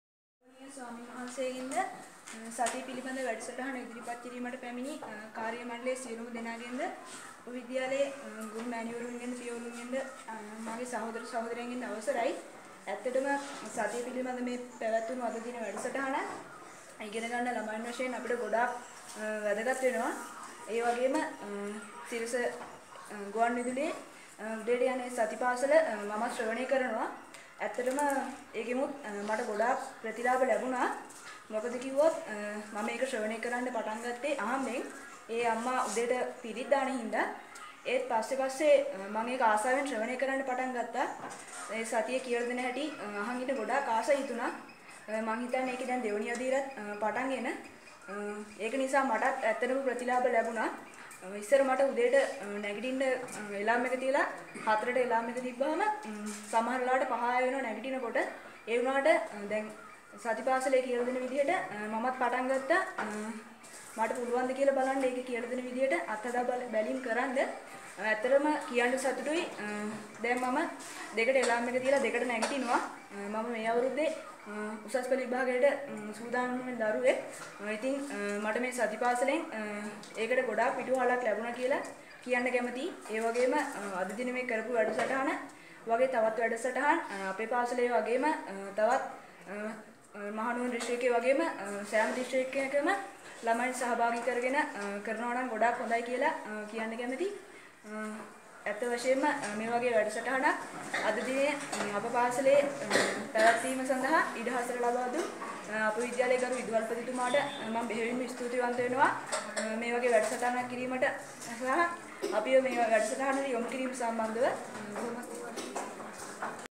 Satipasala at Wimalarathna Kumaragama MV : Appreciation
A Satipasala program was conducted at the Wimalarathna Kumaragama Maha Vidyalaya in Narangpanawa on 22nd of January 2019.